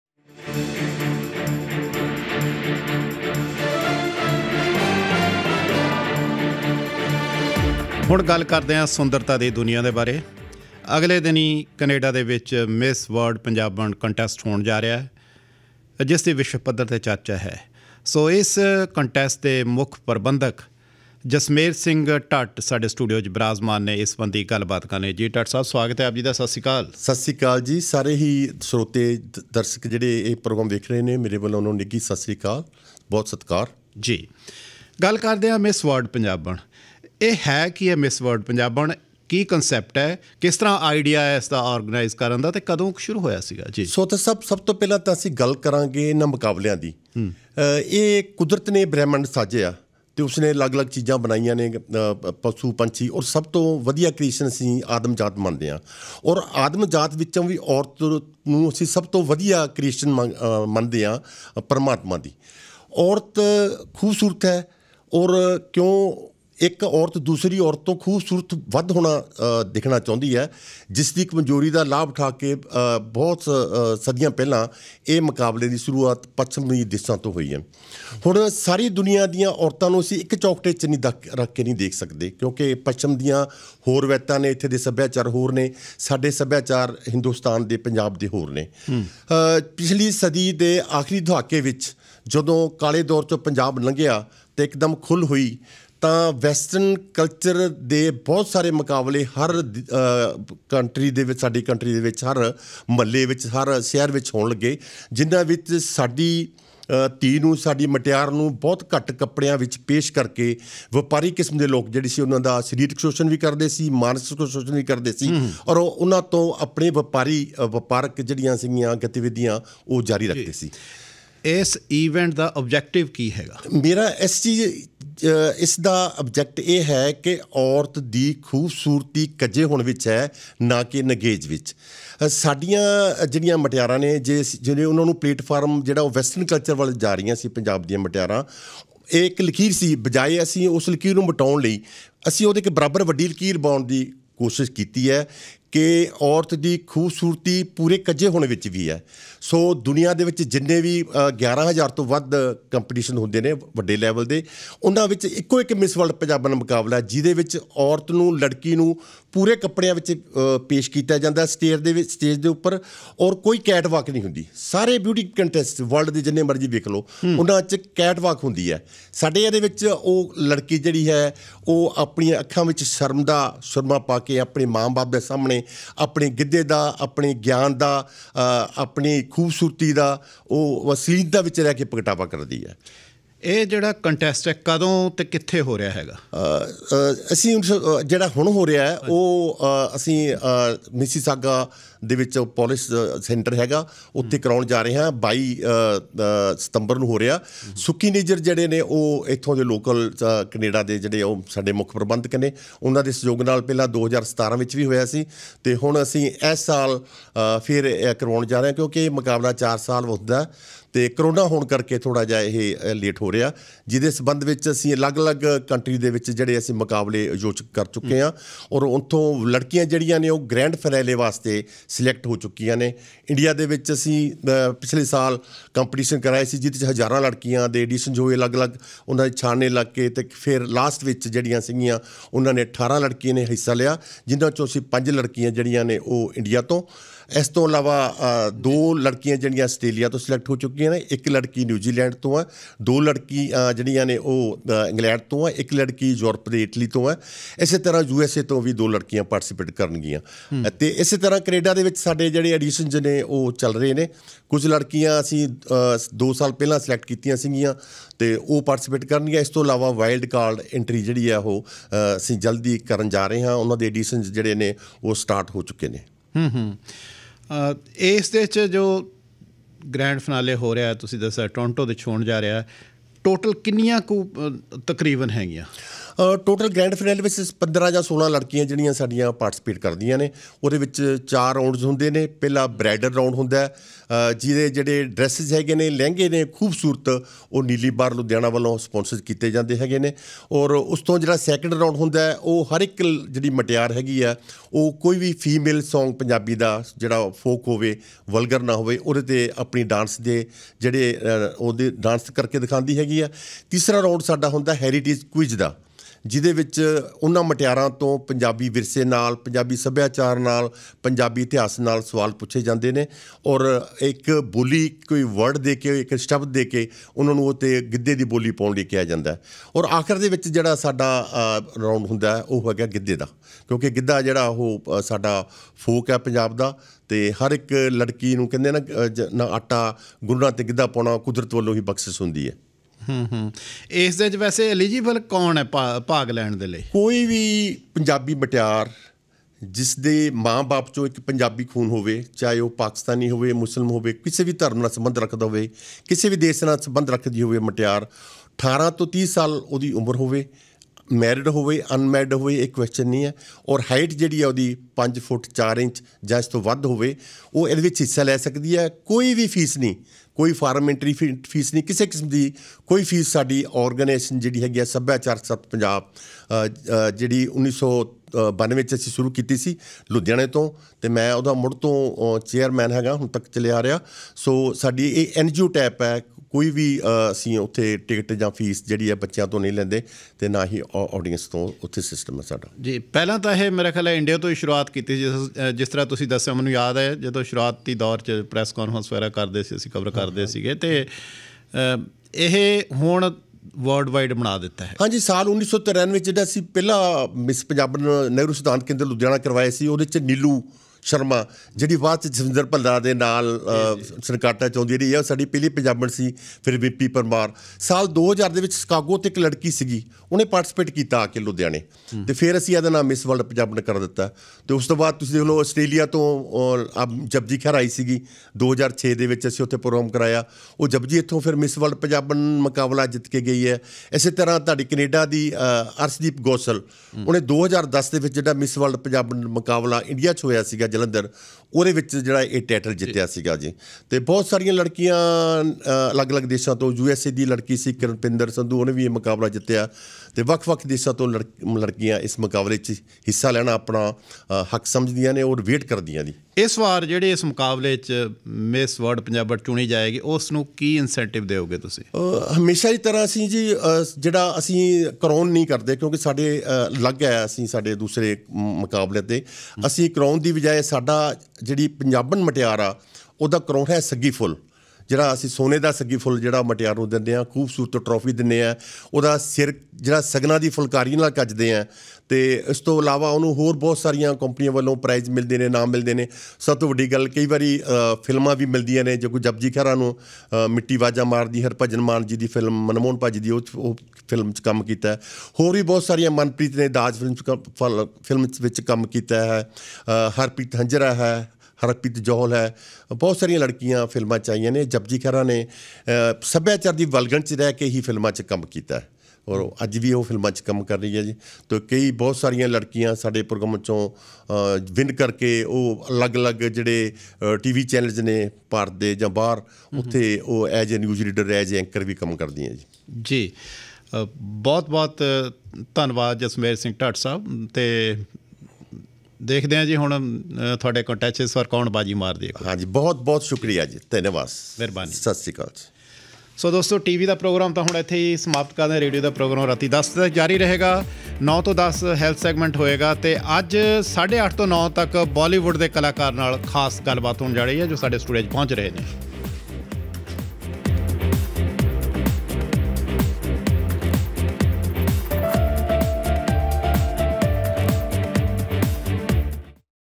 Miss World Punjaban 2023 Details by Jasmer Singh Dhatt
279 Miss World Punjaban 2023 Details by Chairman Jasmer Singh Dhatt on Focus Punjabi Omni News in Edmonton. Miss World Punjaban is being held on 22 September, 2023 at John Paul Polish Cultural Centre, Missisauga, CA.